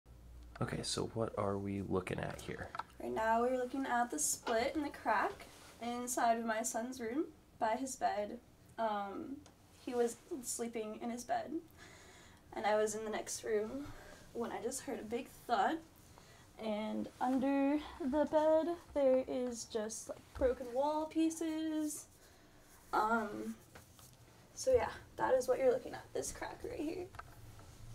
Bend mom shows crack in wall next to son's bed after DUII crash
Michalla Garcia shows the crack in the wall next her her son's bed after Bend Police say a DUII driver hit the outside of the apartment building Wednesday night.